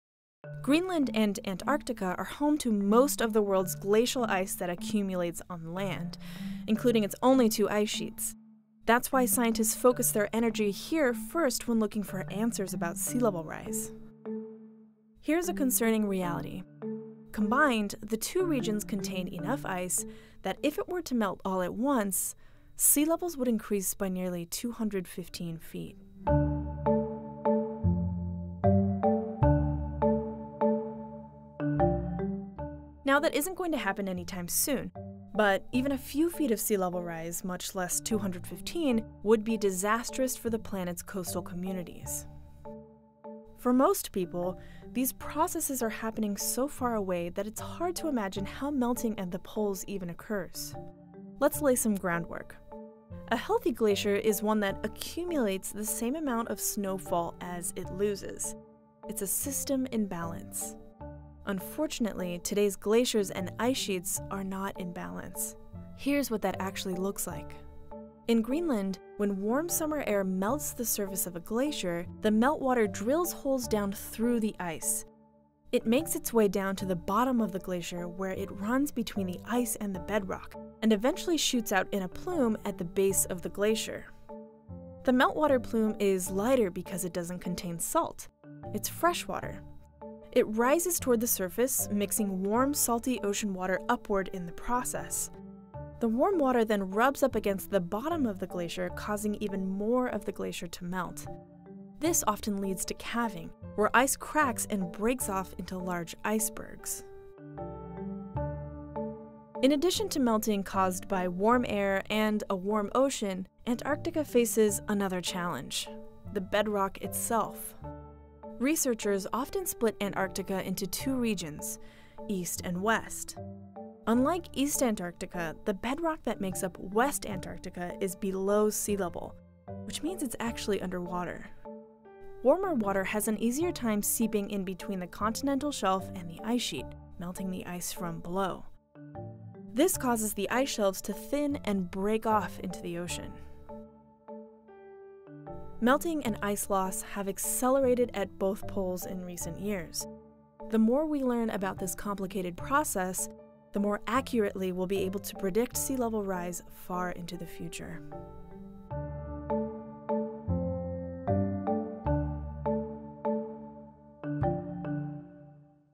Rising Waters: Out-of-Balance Ice Sheets
Greenland and Antarctica are home to most of the world's glacial ice – including its only two ice sheets – making them areas of particular interest to scientists. Combined, the two regions also contain enough ice, that if it were to melt all at once, would raise sea levels by nearly 215 feet (65 meters) – making the study and understanding of them not just interesting, but crucial to our near-term adaptability and our long term survival in a changing world.

Video credit: NASA's Goddard Space Flight Center
LK Ward (USRA): Lead Producer
Esprit Smith (JPL): Lead Writer
Cindy Starr (USRA): Visualizer
Aaron E. Lepsch (ADNET): Technical Support